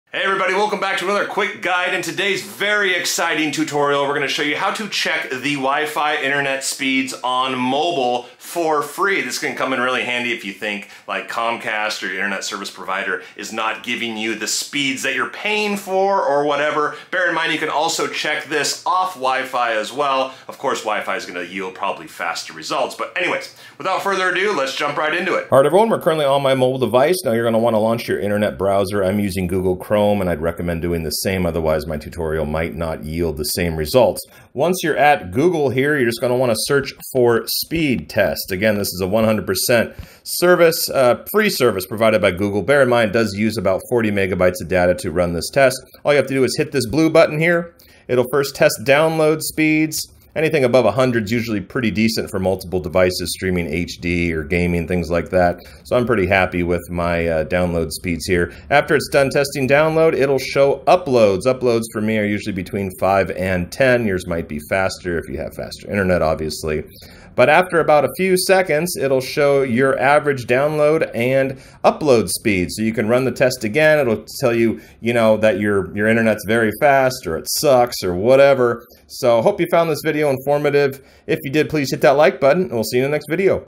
How To Check WIFI Internet Speed on Mobile for FREE!
In today's guide I show you how to check WiFi internet speeds on mobile for free as well as how to check your internet speed and finally test internet speed.

If you are ever curious what your internet speed or WiFi internet speed is, this video should help you!  There are multiple ways to check or test internet speed on mobile but I am going to show you what I think is the easiest and fastest way and it's also completely free through Google.  Bear in mind to run the test, it can use up to 40mb to run the test.

Head over to Google and search for "speed test."  You should see a blue button that says "run speed test."  Simply click on this button and it will run a speed test for both download speeds and upload speeds.  Then it will give you an average and tell you whether you internet is fast or internet is slow and has room for imporvement.

Well everyone, I hope you found today's tutorial helpful!  Please consider giving it a like if this helped!